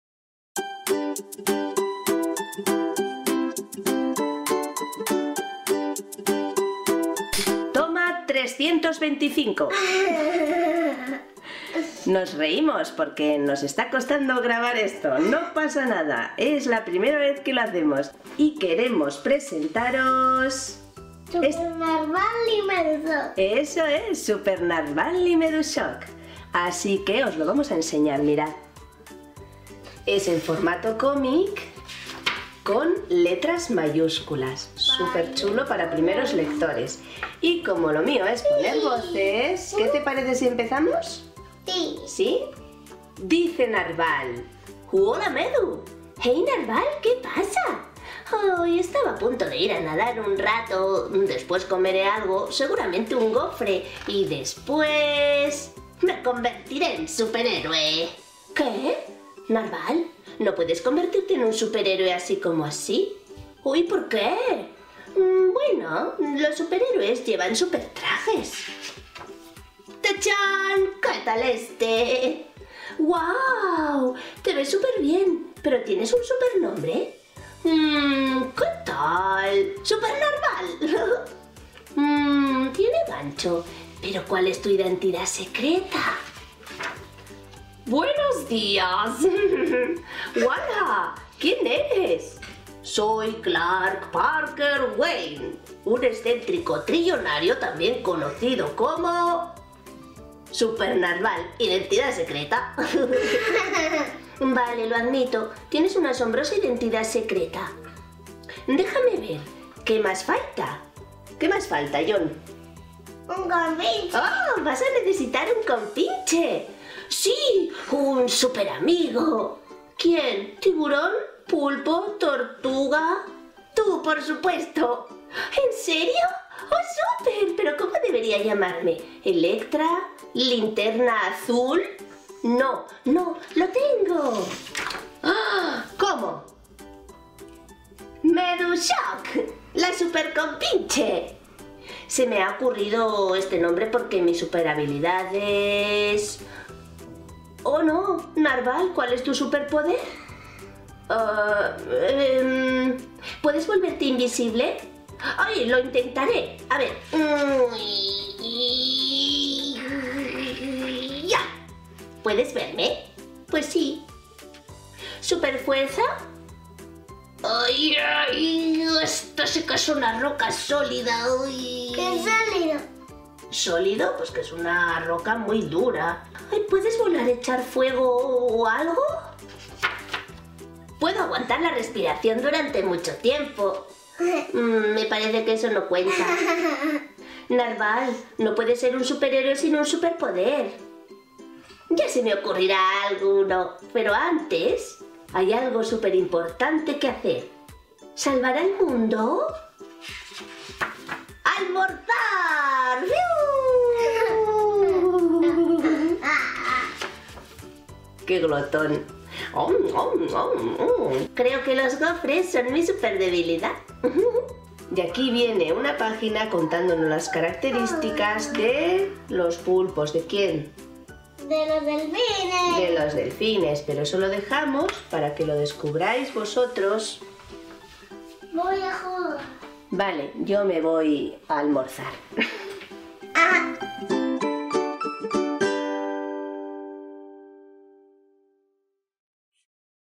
SUPER NARVAL Y MEDU SHOCK (PEQUEÑA LECTURA)
Segundo título de los Libros de Narval y Medu. Una magnífica serie para que los pequeños empiecen a ser lectores autónomos, descubriendo algunas de las infinitas curiosidades del mar y contagiándose de la magia de estos entrañables personajes que no dejarán indiferente a nadie.
En esta ocasión Narval y Medu se convierten en superhéroes, con sus supertrajes, sus identidades secretas ¡y sus superpoderes! Pero ¿cuál será el superpoder de Narval? Disfruta con las aventuras submarinas de esta superpandilla y con las de sus alteregos Supergofre y Wonderfresa.

Texto e ilustraciones: Ben Clanton.
Editorial: Juventud.